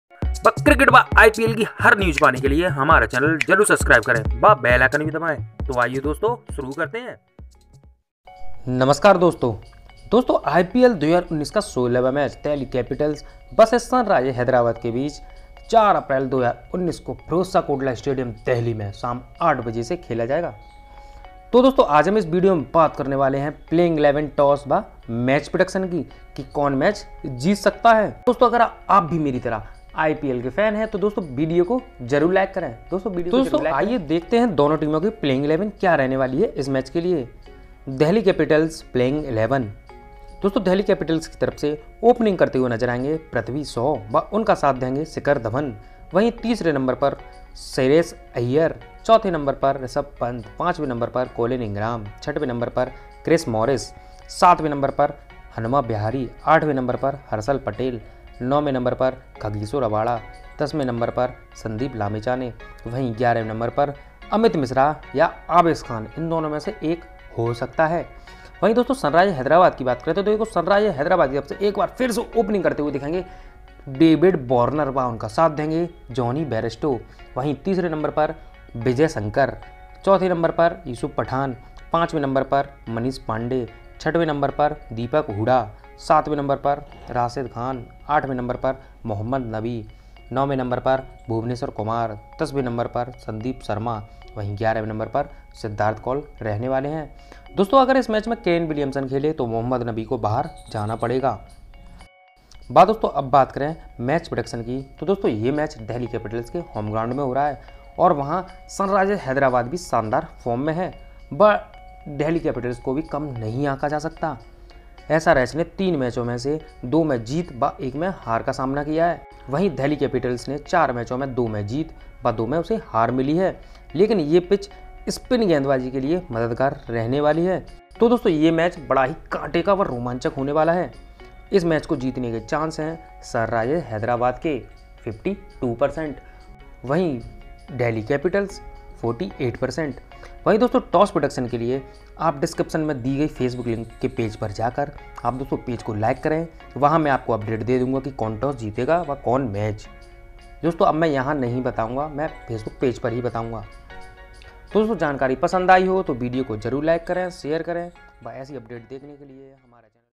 IPL 2019 DELHI CAPITALS VS SUNRISE HYDERABAD PLAYING 11 & MATCH PREDICTION | DC VS SRH | PLAYING XI
टॉस हैदराबाद

टॉस प्रेडिक्शन के लिए नीचे दी गयी लिंक पर क्लिक करे